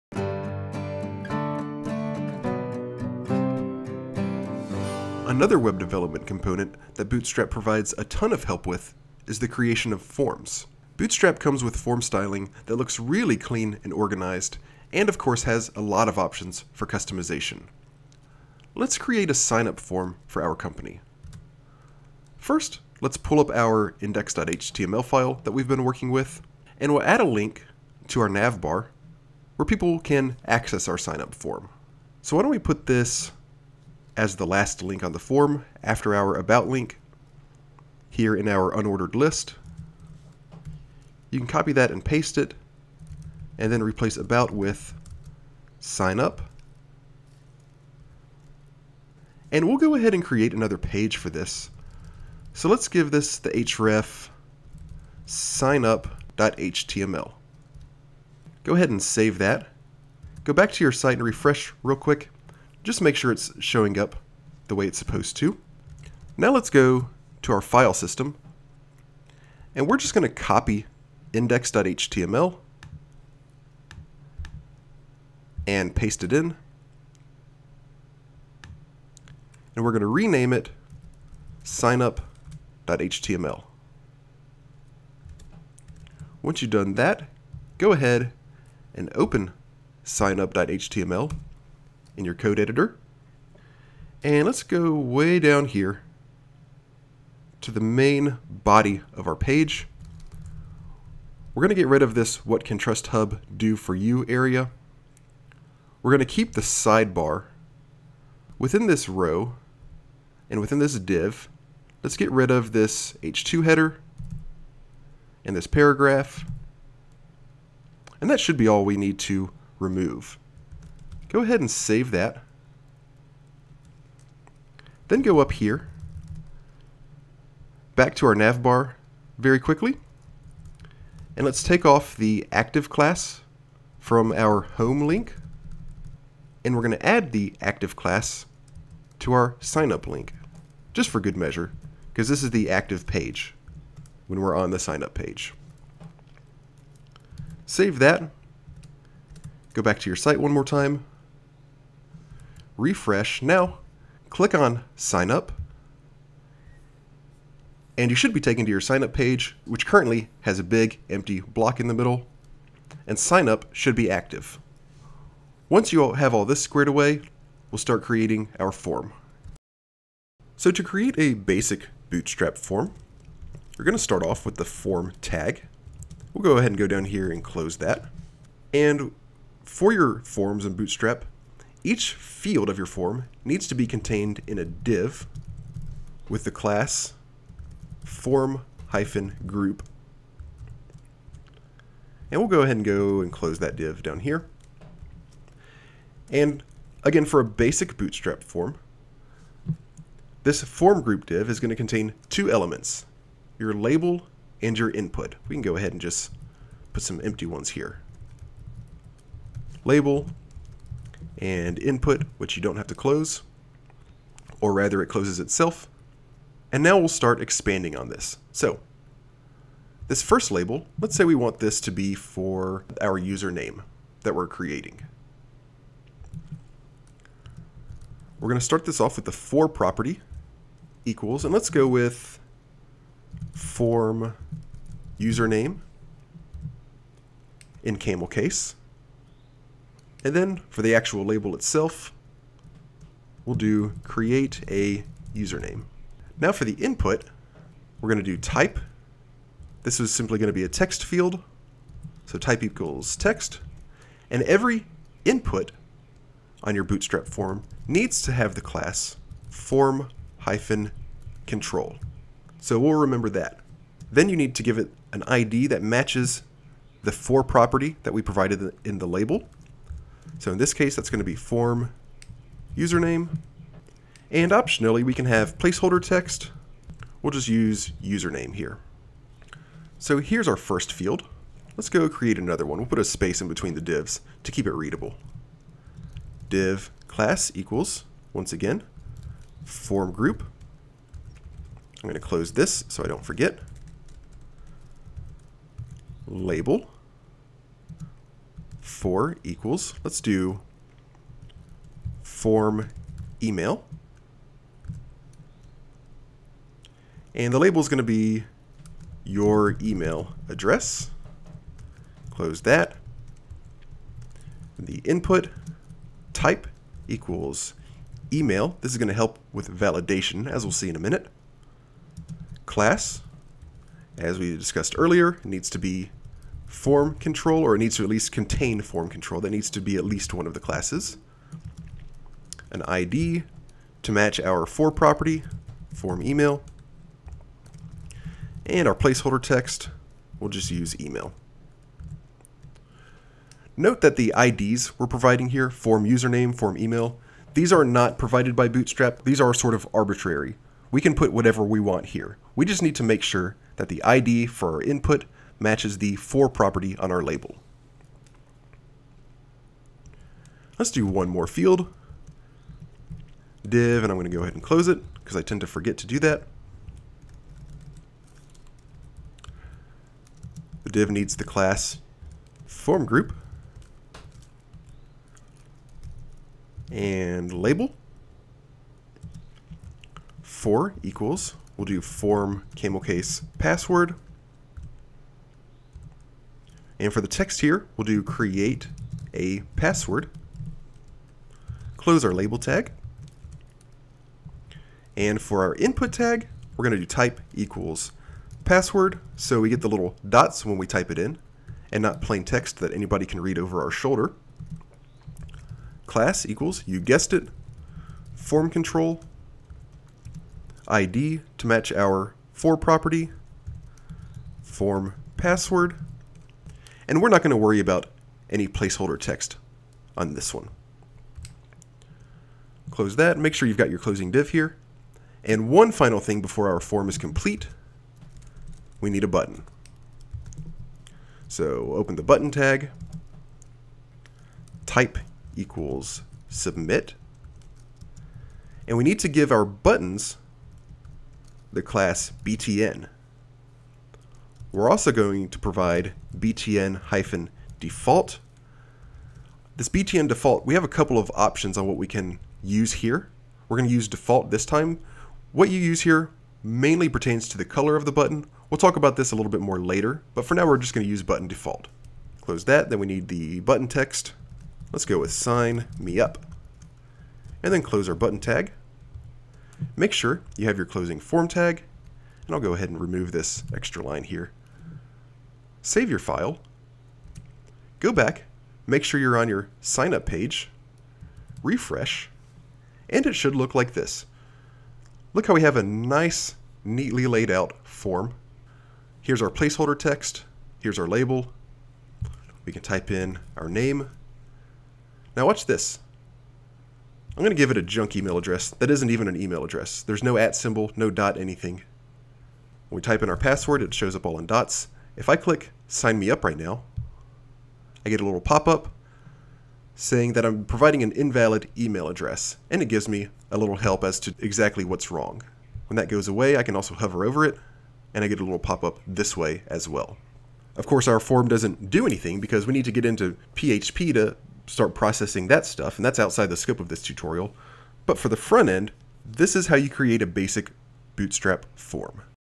How to Create Forms Using Bootstrap
In this video, we are going to create a signup form with Bootstrap. The Bootstrap framework provides a fantastic set of tools for creating forms.